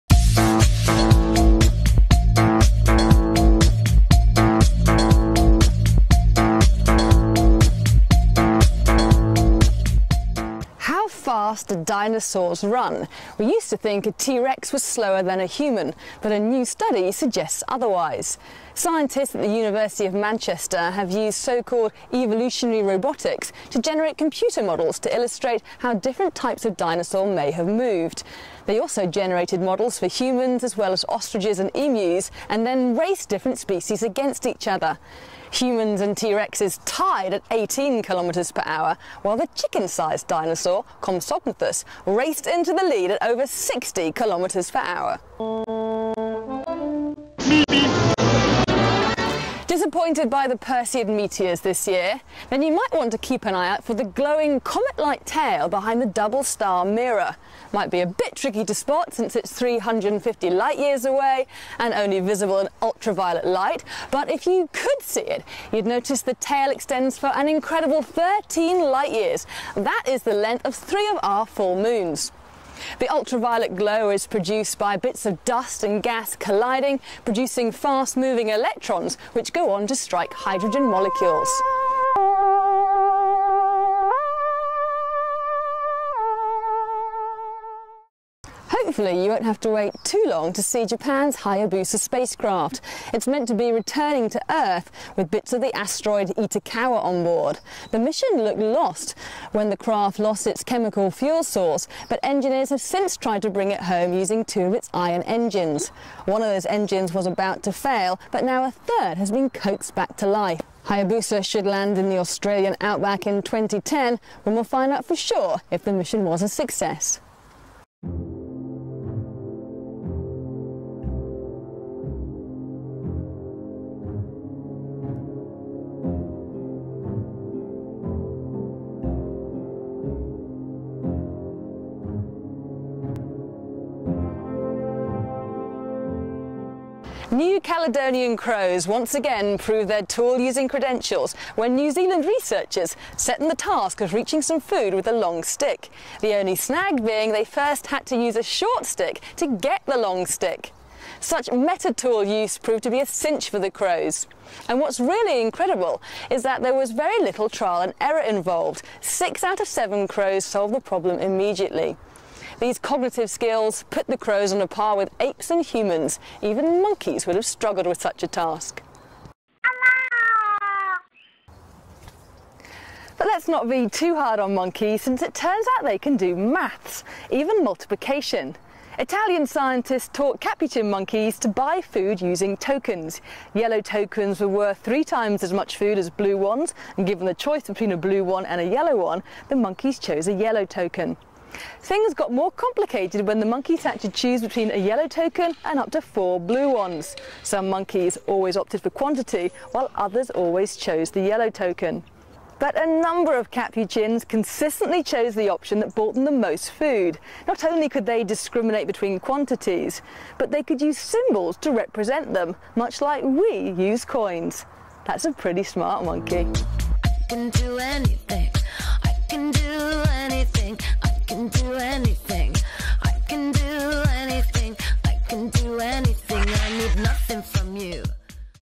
Ep 6 Stillwell Science News
More science news and clips...A Hayabusa in space, counting capuchins, clever corvoids and speedy dinos... Featuring the music of DJ Downfall